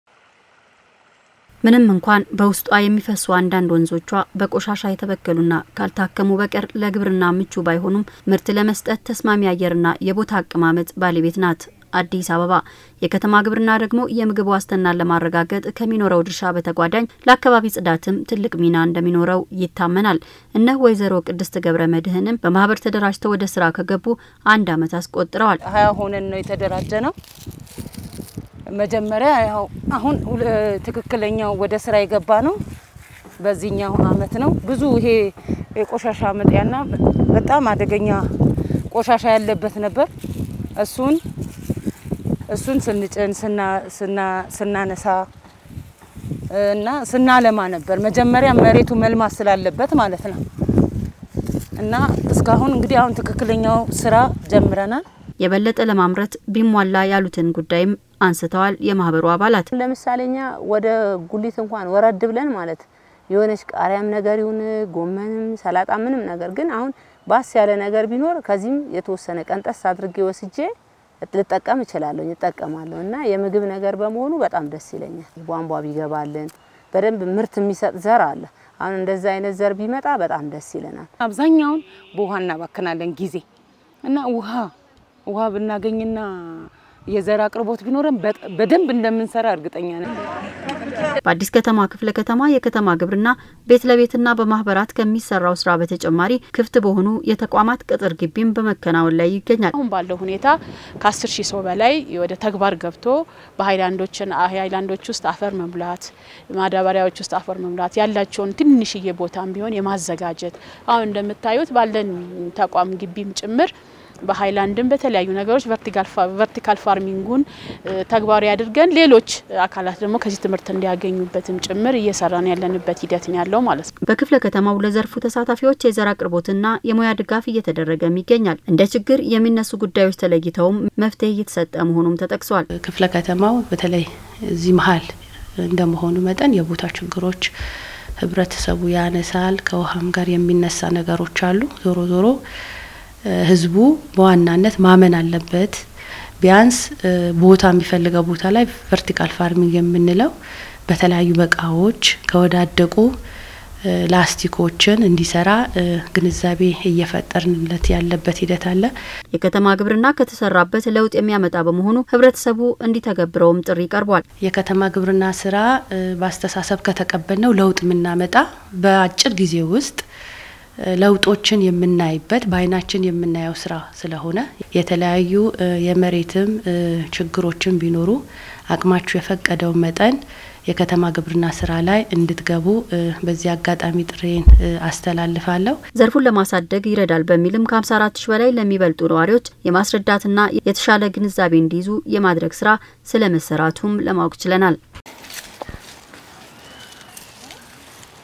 የከተማ ግብርና ተጠቃሚዎች በአዲስ ከተማ
በከተማ ግብርና የተደራጁ ነዋሪዎች በዘርፉ በመሰማራታቸው ተጠቃሚ መሆናቸዉን    መቻላቸውን ገለጹ፡፡
አዲስ ሚዲያ ኔትዎርክ ሰኔ 29/2014 ዓ.ም